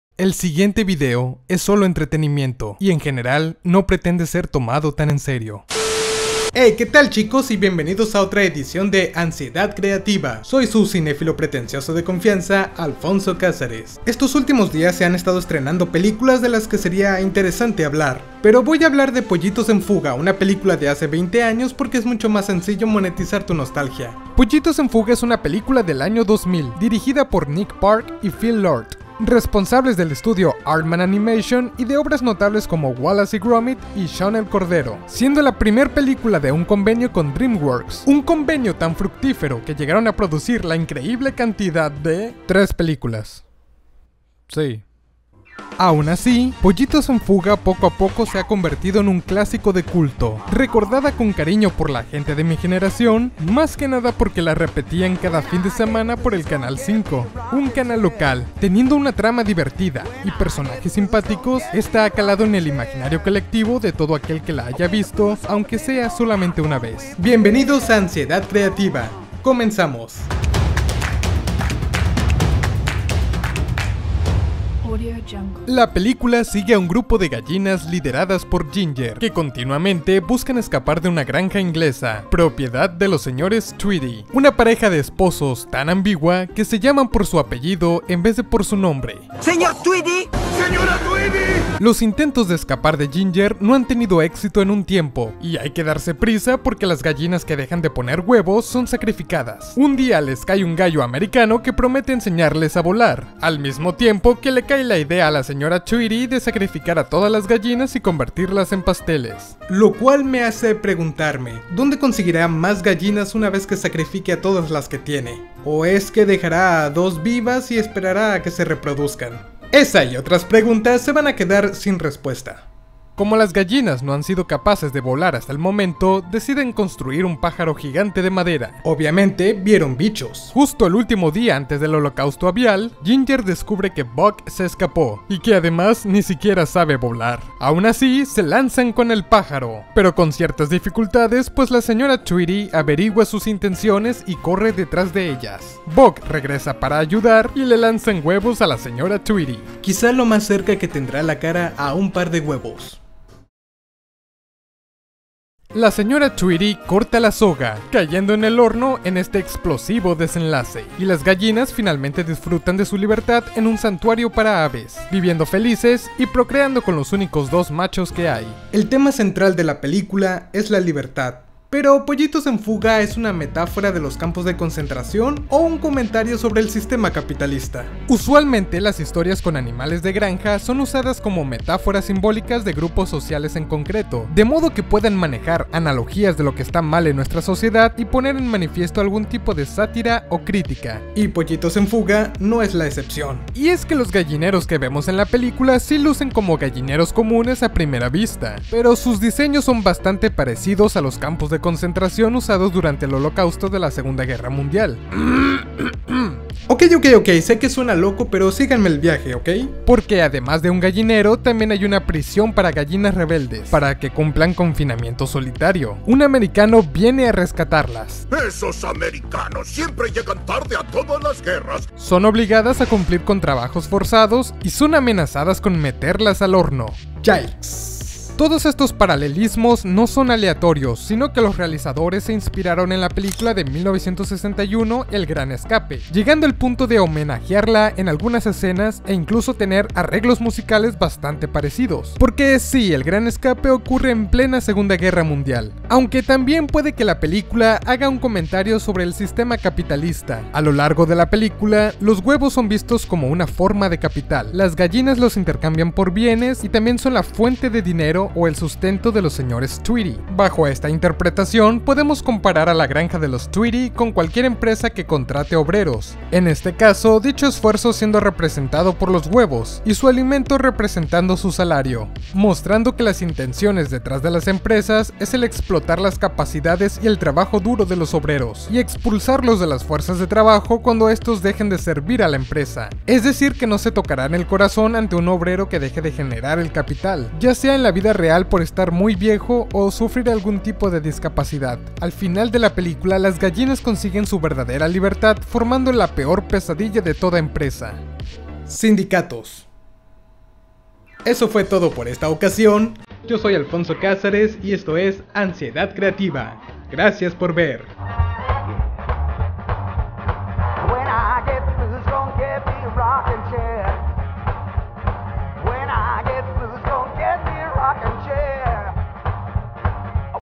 De esto trata POLLITOS EN FUGA en realidad
Los significados ocultos de la película Pollitos en fuga del año 2000.